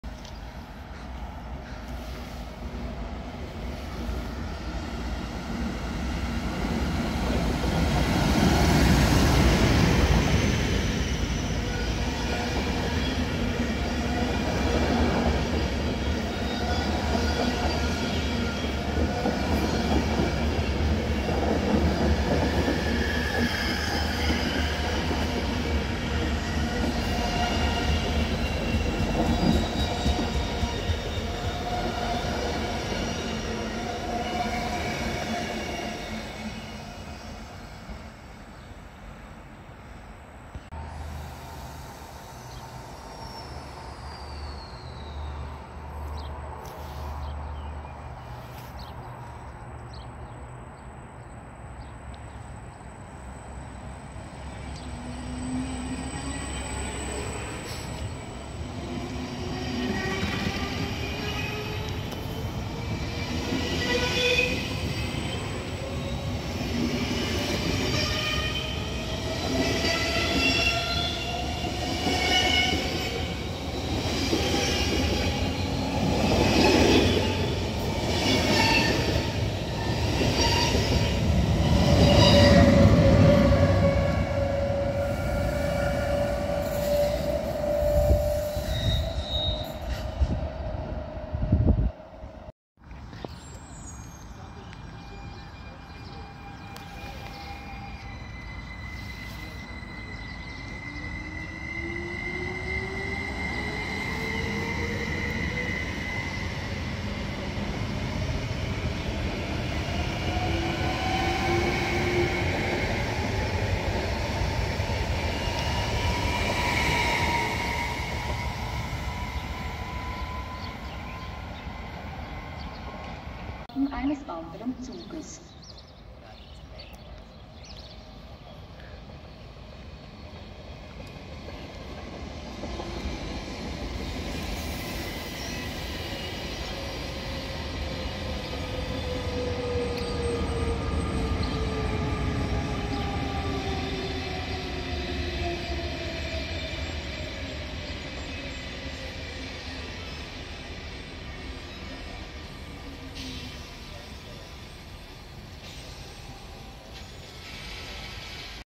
Bahnverkehr in Sargans:
0:00 Re 420 149 mit Nightjet 466 nach Zürich Hb.
0:41 IC nach Chur mit Re 460 063 als Schublok.
1:33 SOB Traverso bei der Ausfahrt.
2:04 IC bei der Einfahrt.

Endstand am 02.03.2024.